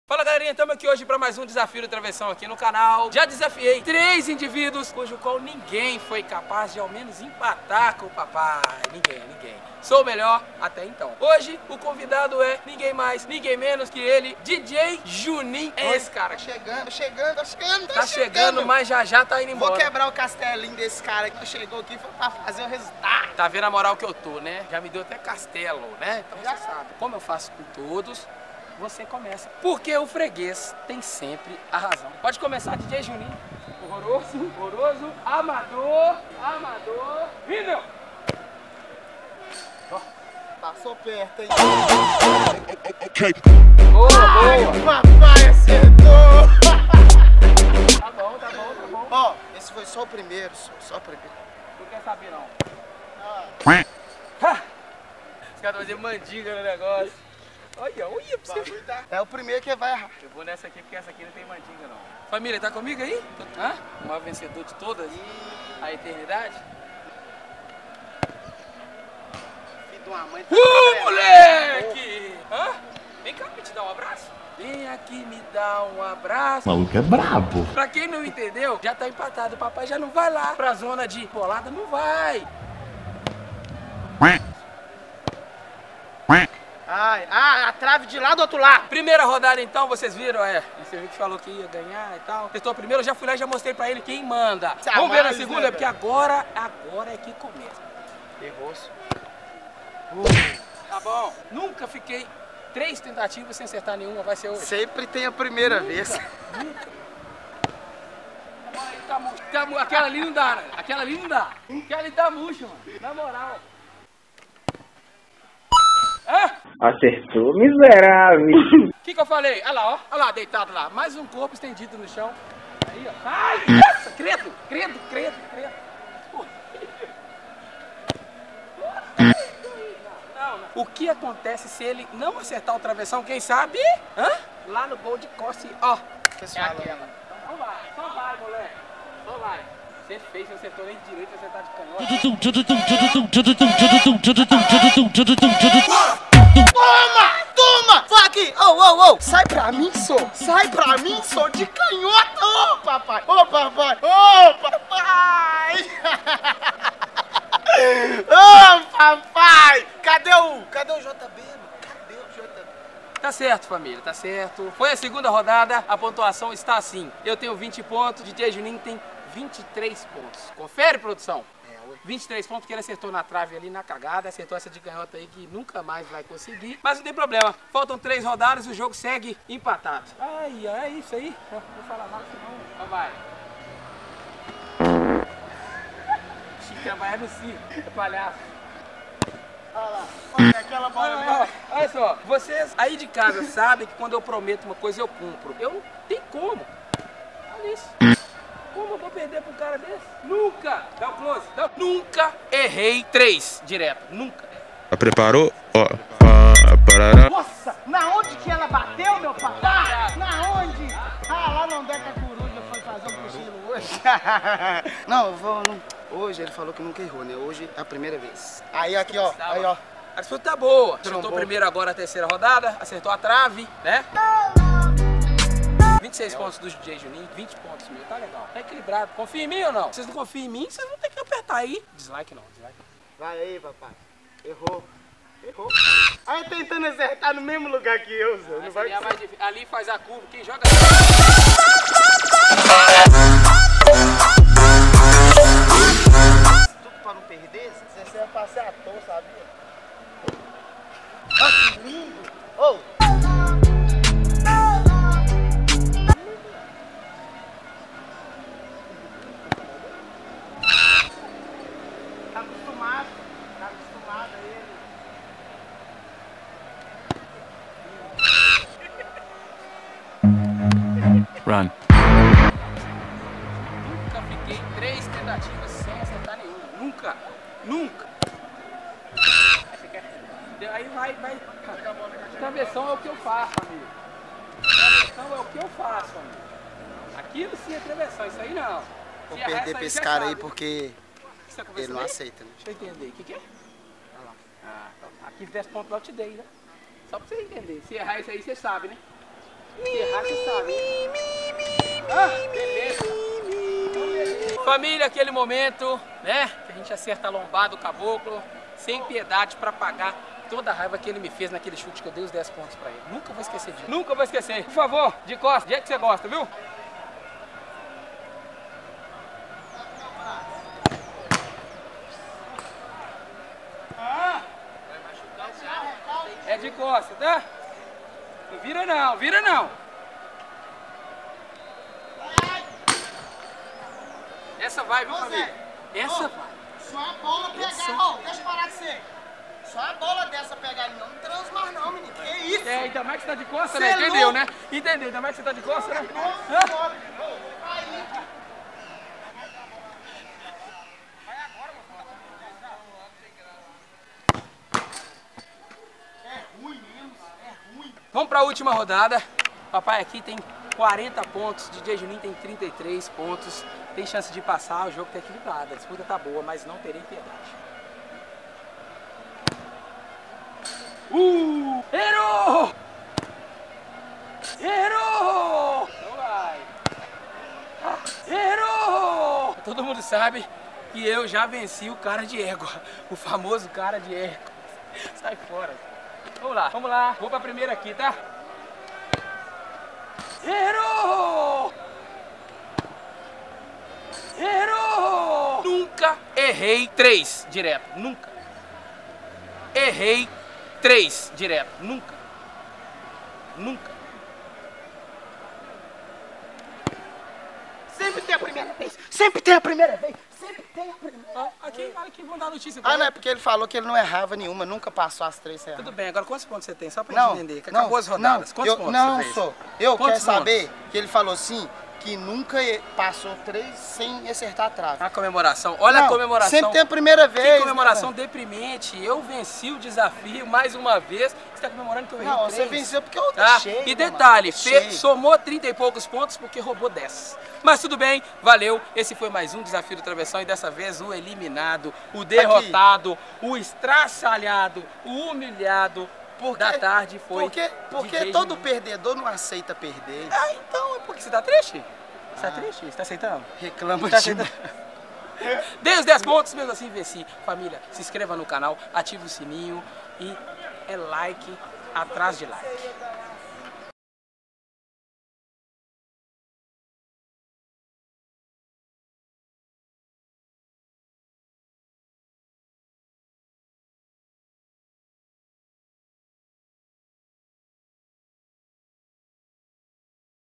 Desafio do Travessão / Dj Junim
Desafio do Travessão com Dj Junim, fizemos uma boa disputa, foi muito difícil, algumas falhas, dúvidas se acertou ou não.
Más no fim sempre vence o Melhor..
assistam e espero que gostem.